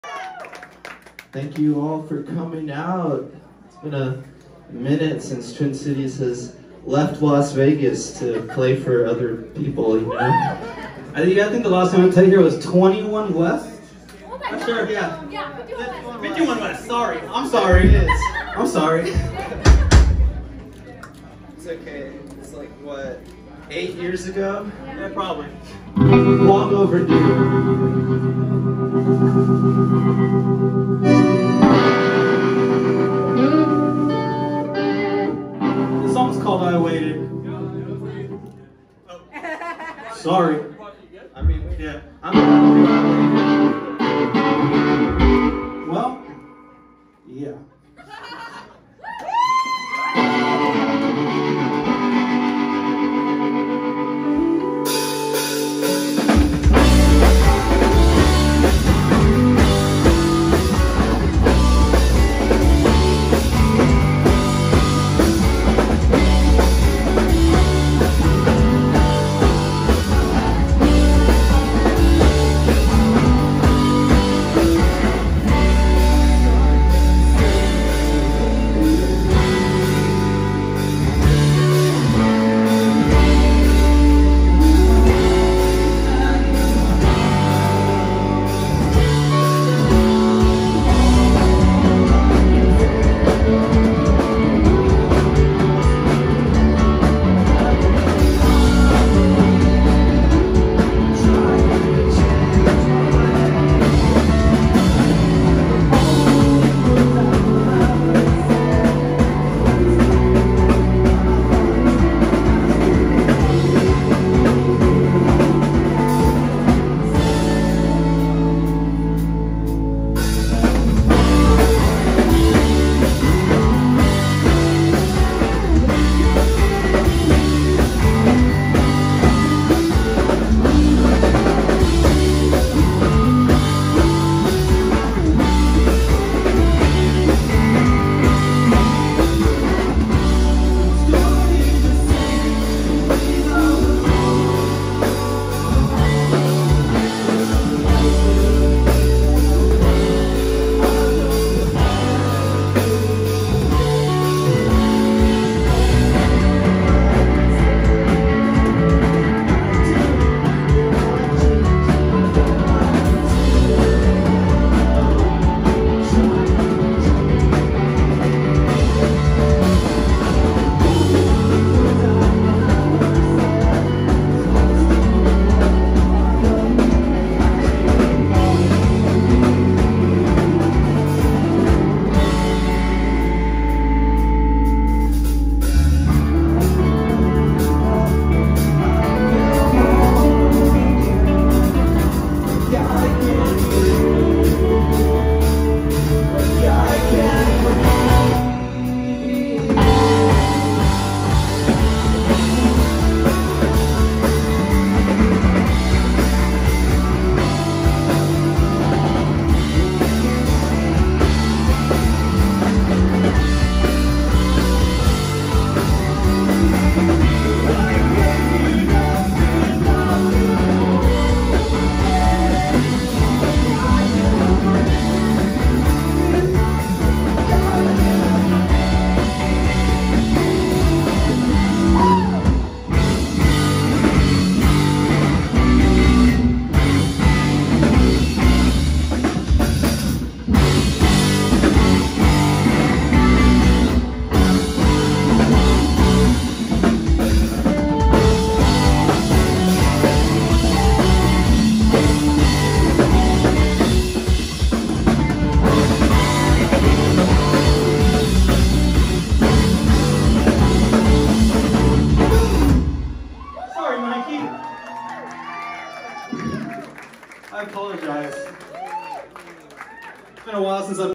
TWIN CITIES (Live at The Nile Theater in Mesa, AZ)
This band hails from Las Vegas, NV.  I fell in love with this group.  The music, the performance was stellar.  Awesome all around fricking show man.  Can't wait to see them again.